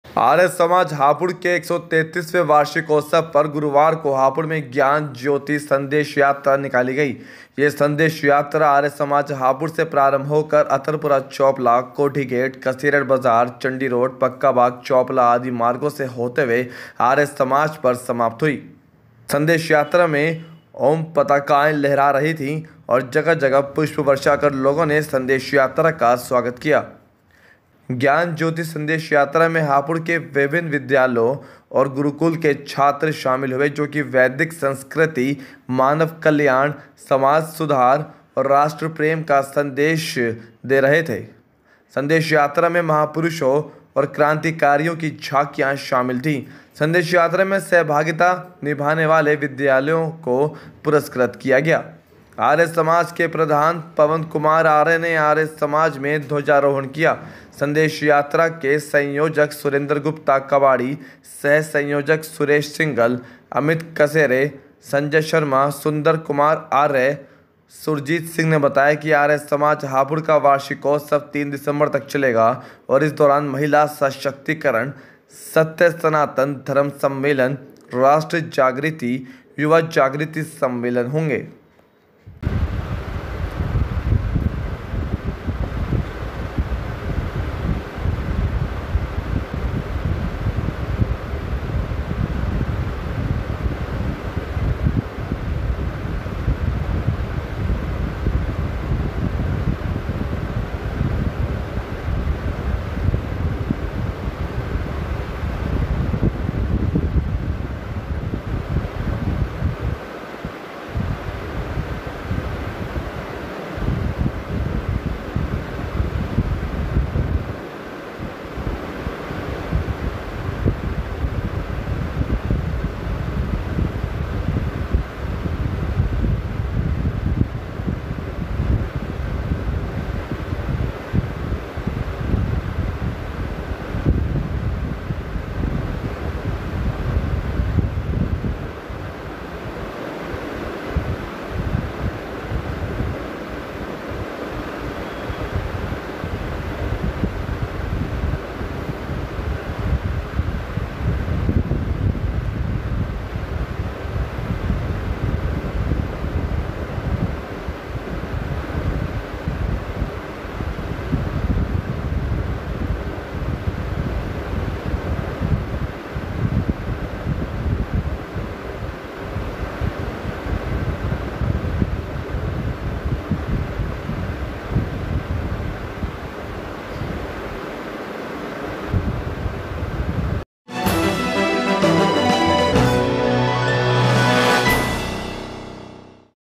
आर्य समाज हापुड़ के वार्षिकोत्सव पर निकली संदेश यात्रा
आर्य समाज हापुड़ के 133वें वार्षिकोत्सव पर गुरुवार को हापुड़ में ज्ञान ज्योति- संदेश यात्रा निकाली गई। यह संदेश यात्रा आर्य समाज हापुड़ से प्रारंभ होकर अतरपुरा चौपला, कोठी गेट, कसेरठ बाजार, चंडी रोड, पक्कबाग चौपला आदि मार्गों से होते हुए आर्य समाज पर समाप्त हुई। संदेश यात्रा में ओऊम् पताकाएं लहरा रही थी और जगह-जगह पुष्प वर्षा कर लोगों ने संदेश यात्रा का स्वागत किया।
ज्ञान ज्योति-संदेश यात्रा में हापुड़ के विभिन्न विद्यालयों व गुरुकुल के छात्र शामिल हुए, जो वैदिक संस्कृति, मानव कल्याण, समाज सुधार तथा राष्ट्र प्रेम का संदेश दे रहे थे। संदेश यात्रा में महापुरुषों एवं क्रांतिकारियों की झाकियां शामिल थी। संदेश यात्रा में सहभागिता निभाने वाले विद्यालयों को पुरस्कृत किया।
आर्य समाज के प्रधान पवन कुमार आर्य ने आर्य समाज में ध्वजारोहण किया। संदेश यात्रा के संयोजक सुरेंद्र गुप्ता कबाड़ी, सह संयोजक सुरेश सिंहल, अमित कसेरे, संजय शर्मा, सुंदर कुमार आर्य व सुरजीत सिंह ने बताया कि आर्य समाज हापुड़ का वार्षिकोत्सव 3 दिसम्बर तक चलेगा और इस दौरान महिला सशक्तीकरण, सत्य सनातन धर्म सम्मेलन, राष्ट्र जागृति, युवा जागृति सम्मेलन होंगे।